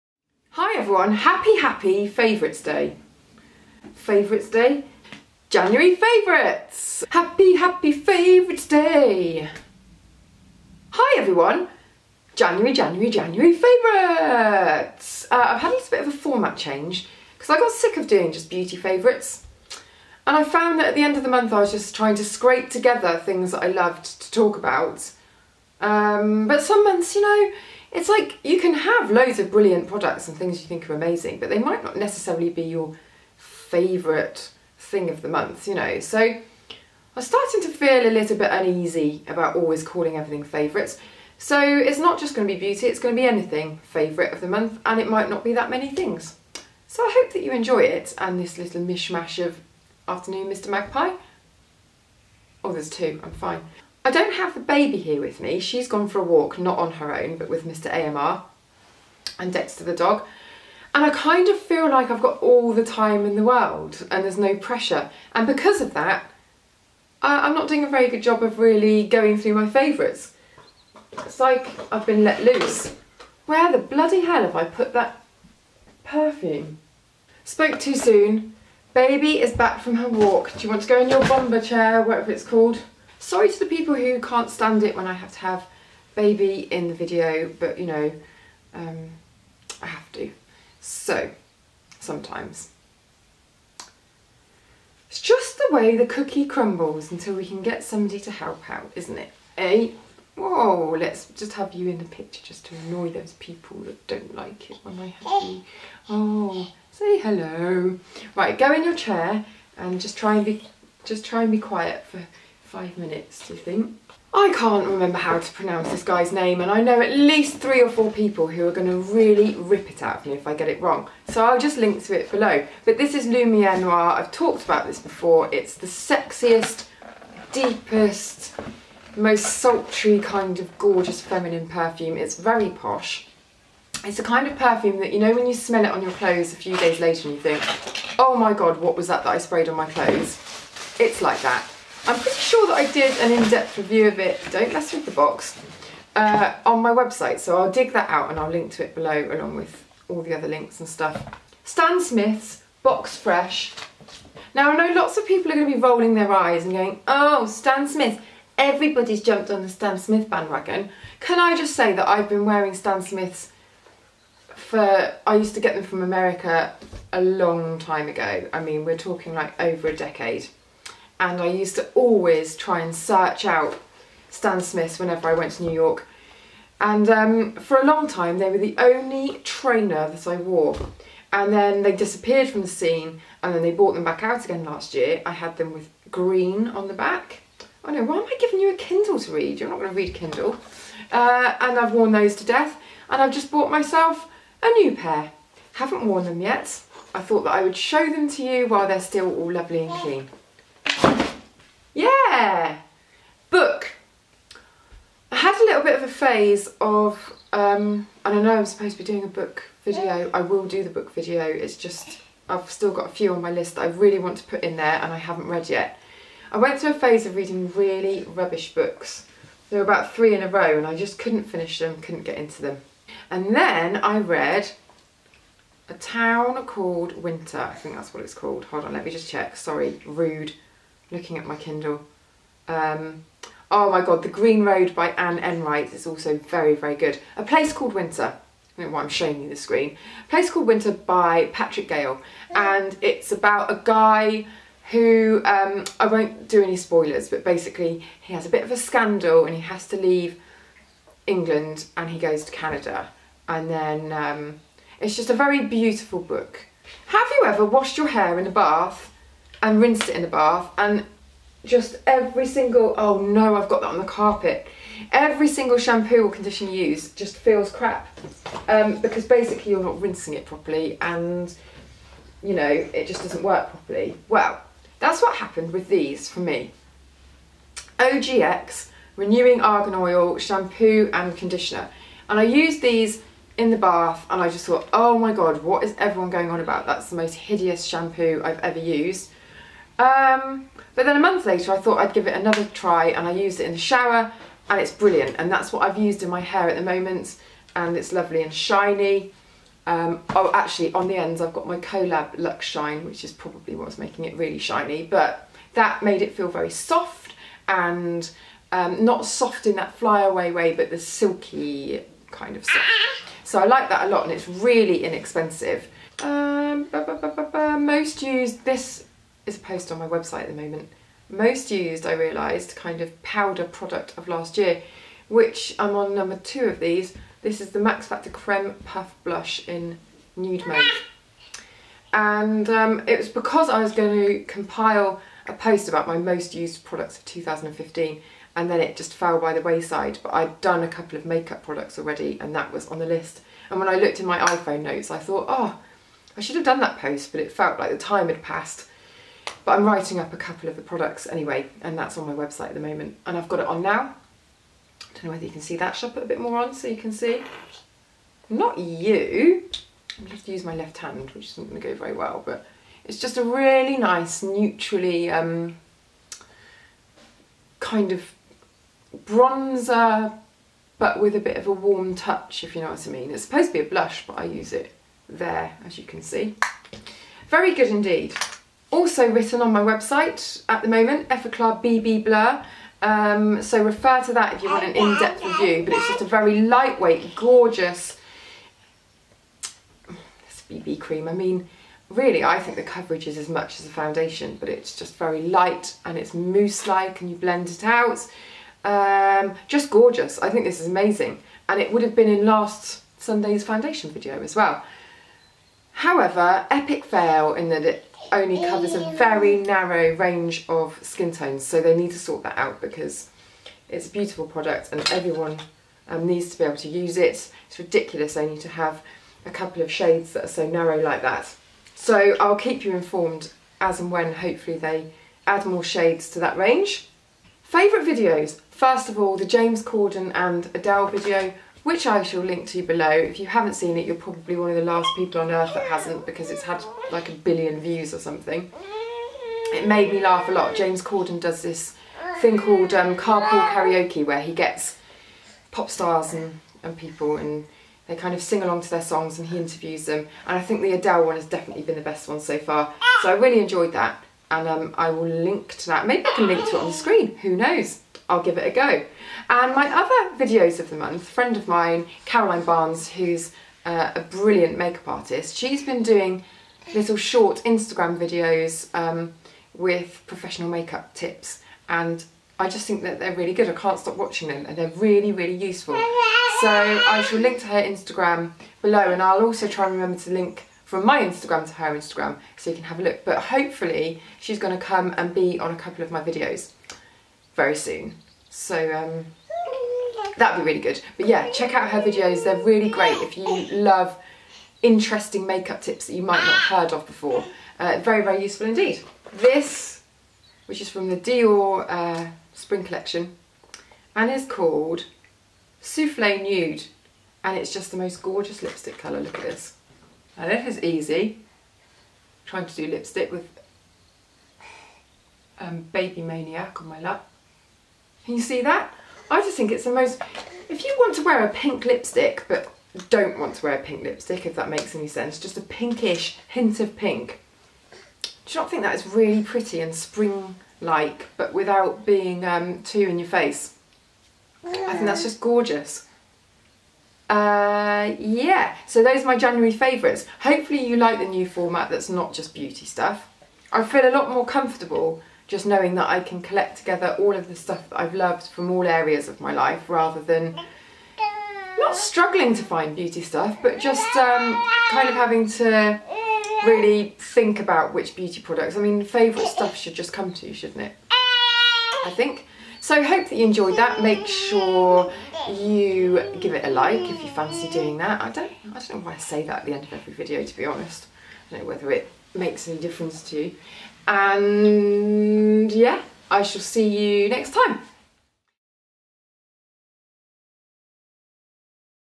January Favourites 2016 | A Model Recommends
Hello! January favourites for you - not just beauty, from now on, it's my favourite things from ALL parts of my life! Hope that you enjoy it.
Ruth xx